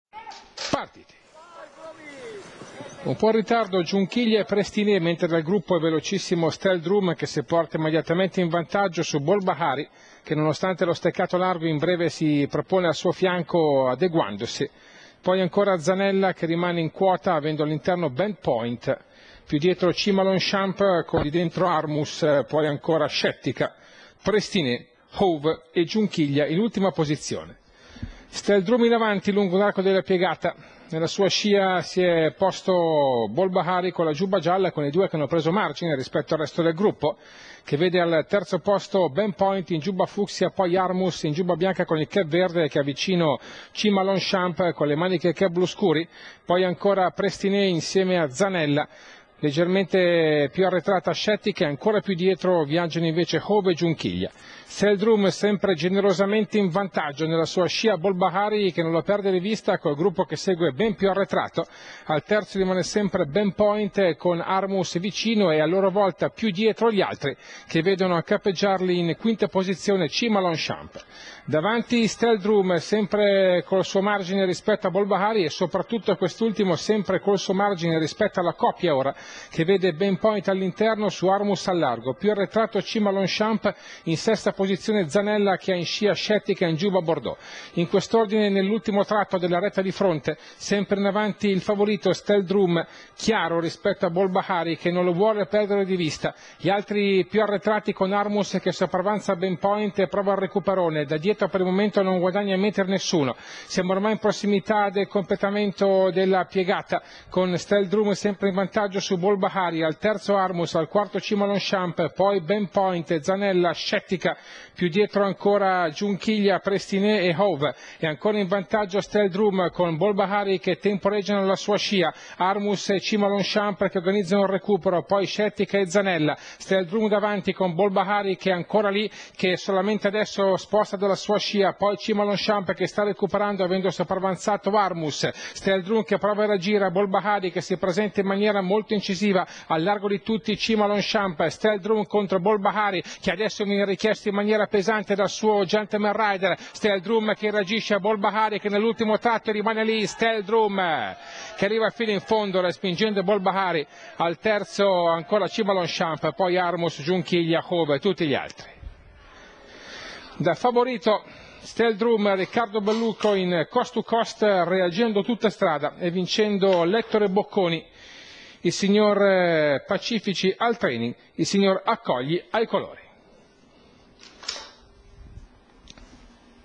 07/06/2014, Milano , Premio Ettore Bocconi
1. STELL DRUM (BELLUCO RICCARDO), 2. BOLD BAHARI (FROMMIJA FEDERICO), 3. CIMA LONGCHAMP (GRASSO CAPRIOLI V.), 4. ARMUS (MARIA MONETA), 5. GIUNCHIGLIA (DE ROSSI R.), 6. HOVE (D' ALO' DOMENICO), 7. SCETTICA (LUPINACCI ANNA), 8. ZANELLA (BESANA A.), 9. BEND POINT (LAURA DELL'ELCE), 10. PRESTINEE (PEDERSOLI GRAZIANO)